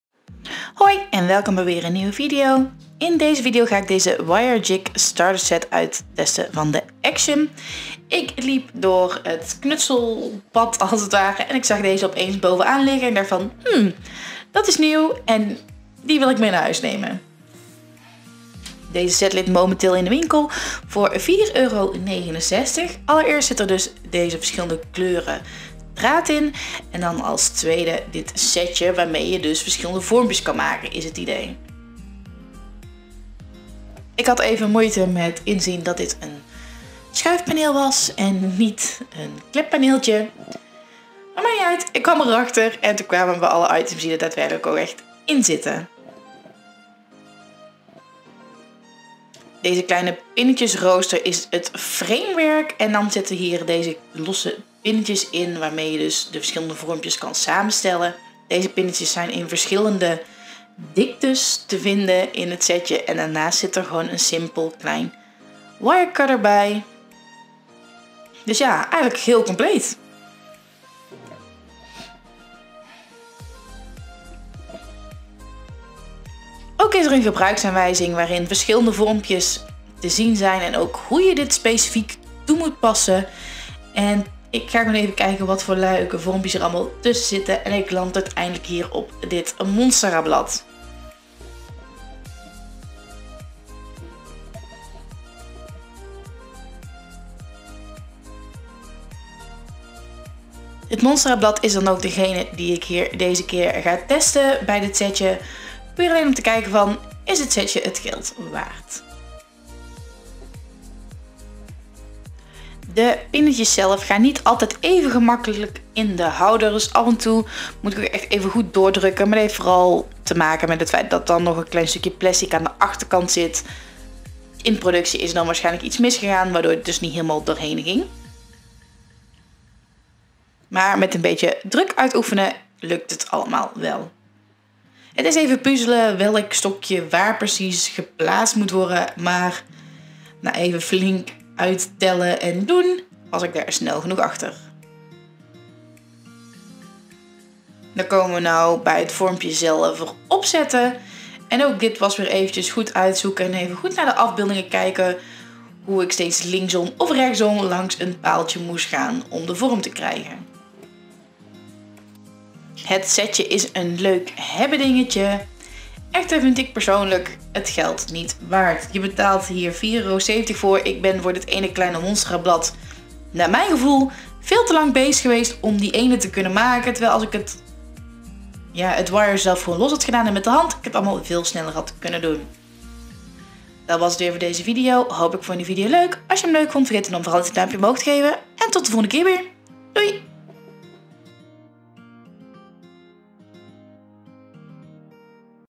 REVIEW || Wirejig set action
In deze video test ik de  wire jig set uit van de Action. Deze ligt momenteel in de winkel voor € 4,69. Kijk met me mee hoe ik deze set voor het eerst gebruik en mijn first impressions hierover geef. En het belangrijkste dat jullie zelf kunnen zien of jullie dit wel of niet moeten aanschaffen.